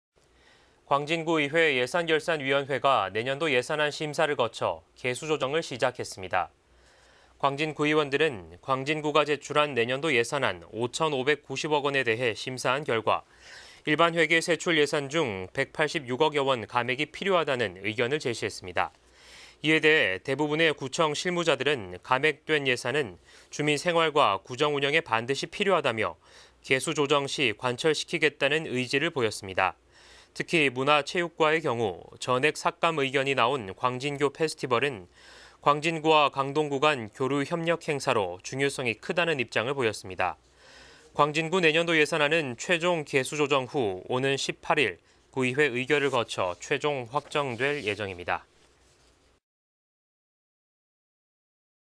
[광진] 광진구의회, 내년도 예산안 계수조정 돌입
【 앵커멘트 】
광진구의회 예산결산위원회가
내년도 예산안 심사를 거쳐
계수조정을 시작했습니다.

【 VCR 】
광진구의원들은
광진구가 제출한 내년도 예산안
5천 5백 90억 원에 대해 심사한 결과
일반회계 세출예산 중
186억여 원 감액이 필요하다는
의견을 제시했습니다.

이에 대해 대부분의 구청 실무자들은
감액된 예산은 주민 생활과 구정 운영에
반드시 필요하다며
계수 조정 시 관철시키겠다는
의지를 보였습니다.

특히 문화체육과의 경우
전액 삭감 의견이 나온
광진교 페스티벌은
광진구와 강동구 간 교류 협력 행사로
중요성이 크다는 입장을 보였습니다.

광진구 내년도 예산안은 최종 계수조정 후
오는 18일 구의회 의결을 거쳐
최종 확정될 예정입니다.